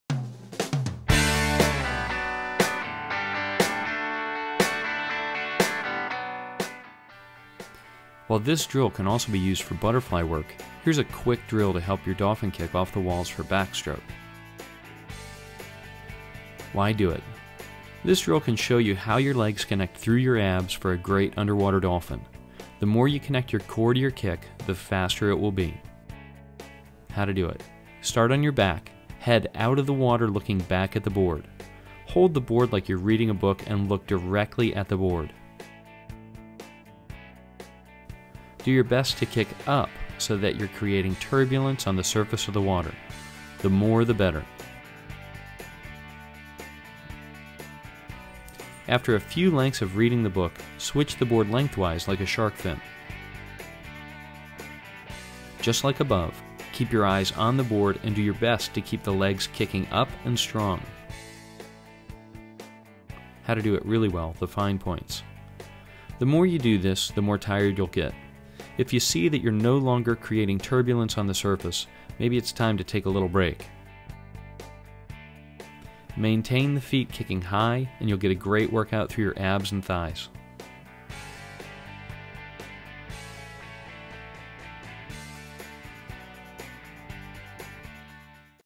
Backstroke - Backwards Board Kick
While this can also be used for butterfly work, here's a quick drill to help with your dolphin kick off the walls for backstroke. Why do it: This drill can show you how your legs connect through your abs for a great underwater dolphin. The more you connect your core to your kick, the faster it will be.